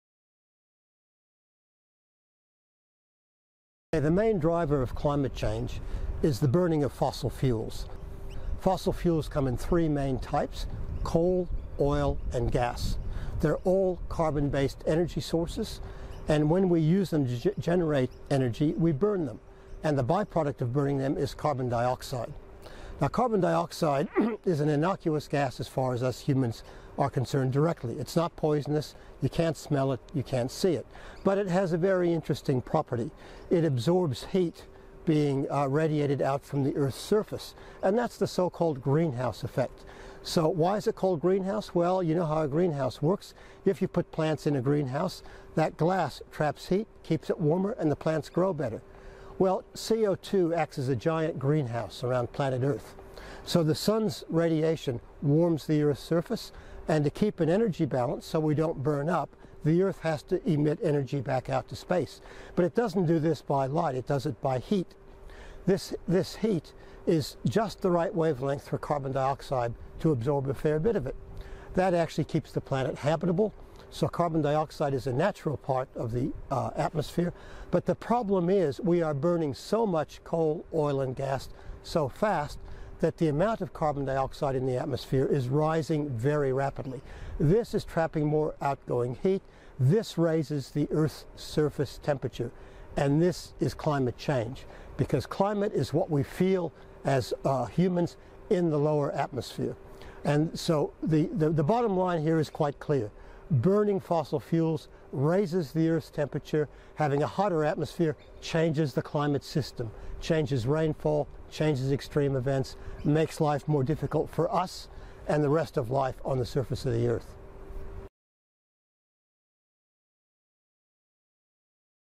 A summary of carbon and climate change | Earth Hazards | meriSTEM
Emeritus Professor Will Steffen from the Australian National University (anu.edu.au) summarises carbon dioxide in our atmosphere and how it's affecting climate change This as part of a series on how humans effect our climate.

This clip has been created by the Research School of Earth Sciences (rses.anu.edu.au). These resources are part of a comprehensive program, curriculum matched to the unit 'The changing Earth - the cause and impact of Earth hazards'.

This video touches on the following curriculum descriptors:
Human activities, particularly land-clearing and fossil fuel consumption, produce gases (including carbon dioxide, methane, nitrous oxide and hydrofluorocarbons) and particulate materials that change the composition of the atmosphere and climatic conditions (for example, the enhanced greenhouse effect) (ACSES105)